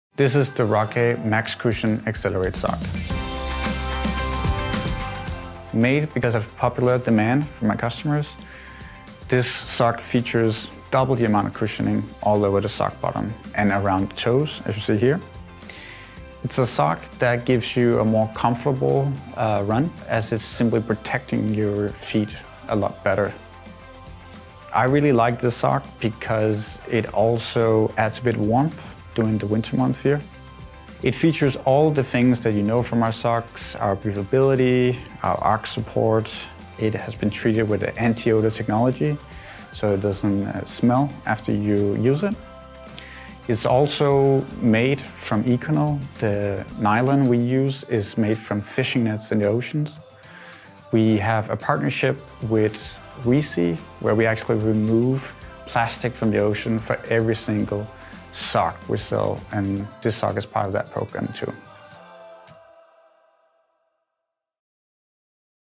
Rockay Accelerate MaxCushion Running Socks
The award-winning Accelerate, packed with strategically placed maximum cushioning, giving you extra comfort and support when you need to power through a demanding workout. Added anti-odor technology and sweat-wicking fabrics helps keep your feet dry and fresh. A true ankle sock, made to perform.